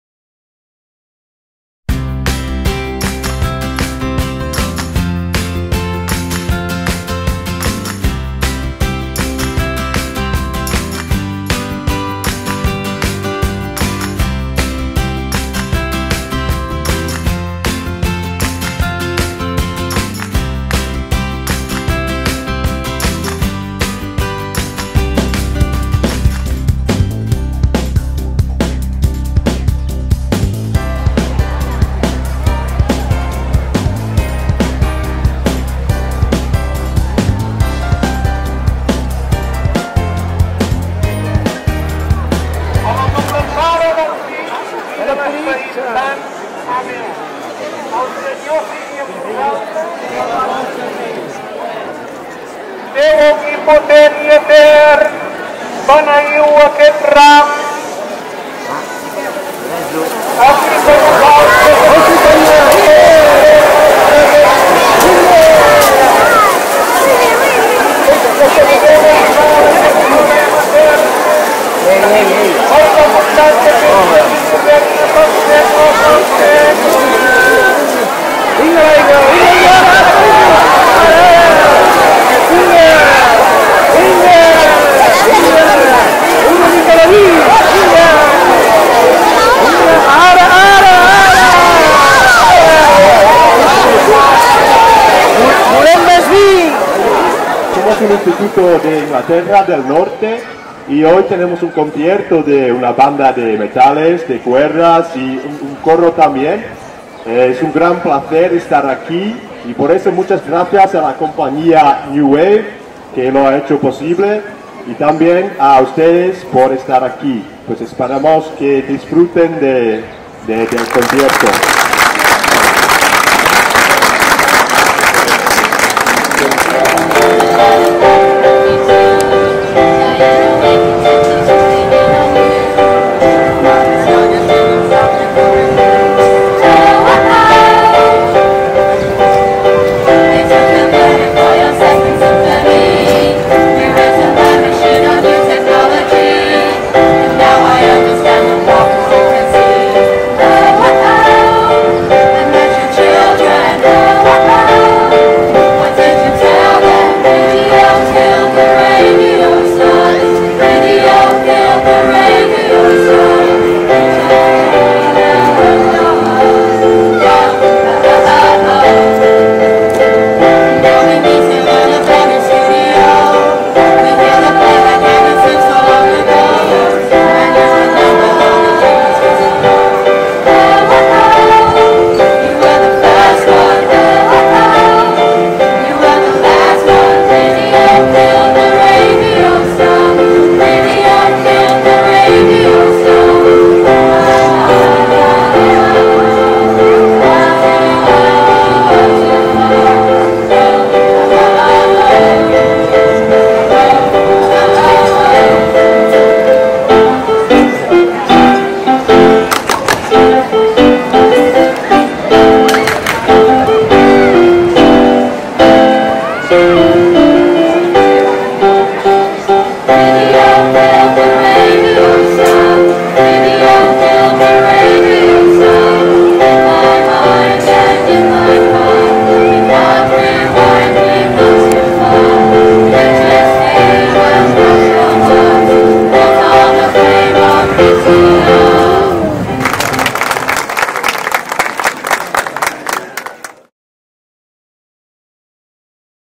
Calafell tot l´any, cultura i tradició.
Inici de la Setmana Santa a Calafell